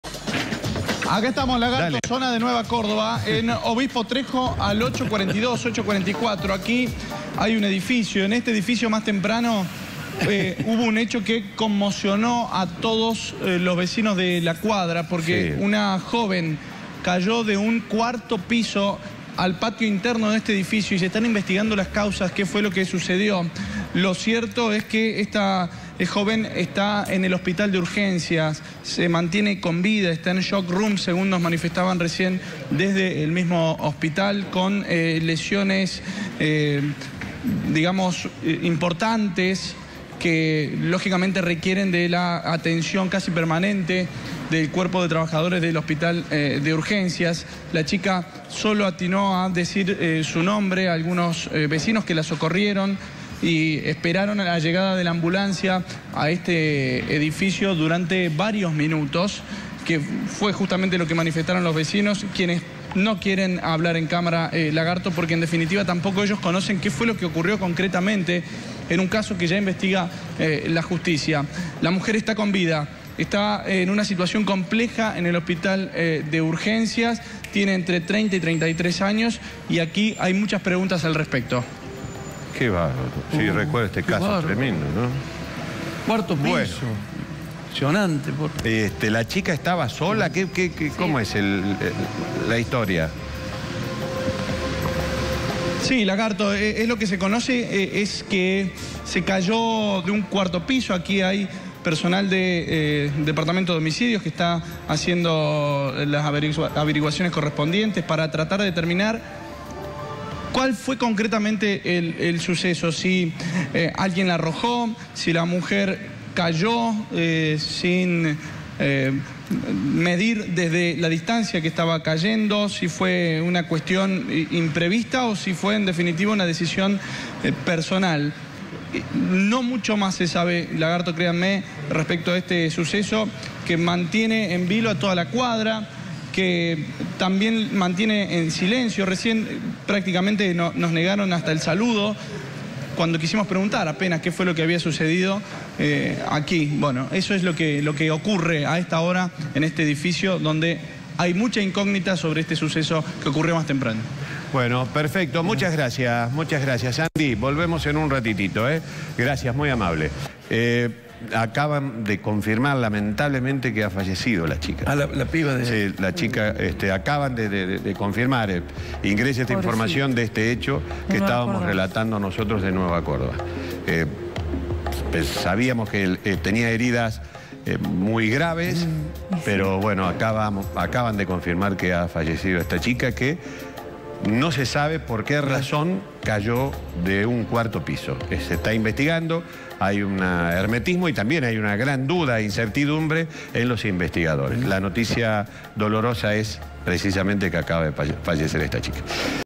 Investigan muerte de joven caída desde un 4° piso
La Justicia investiga las causas por las que una joven cayó desde el cuarto piso de un edificio ubicado en Obispo Trejo 844. Los vecinos atinaron a escuchar solamente el nombre de la chica pero se desconoce si fue una decisión personal. La joven fue internada de urgencia y terminó falleciendo consecuencia de las graves heridas.